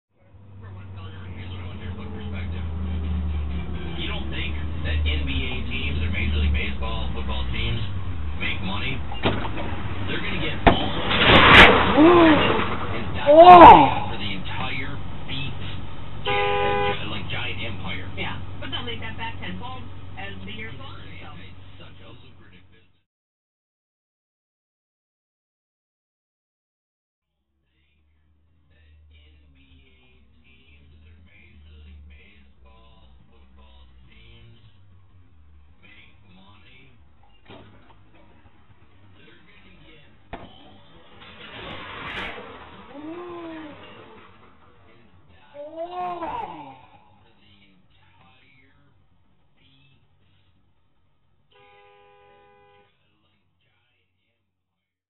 State Patrol Locates Driver of Semi Involved in Dangerous School Bus Incident
UPDATE:
The driver of the semi that illegally passed a school bus and was caught on video turned himself in Tuesday night. Thank you to everyone who shared this video and helped spread the word.

ORIGINAL POST:
The Minnesota State Patrol is looking for the public's help tracking down a semi that illegally passed a stopped school bus May 30, nearly hitting a young female student waiting alongside the highway.

Cameras inside the bus captured the close call, which happened on Highway 23 near Paynesville and New London. Footage shows the semi blow past the stopped bus — lights flashing and stop arm down — and continue along the highway. The girl and bus driver were not injured.

Several witnesses called 911 but were unable to provide enough details for authorities to locate the semi. Anyone with information about this incident or the semi is asked to call Minnesota State Patrol Lt.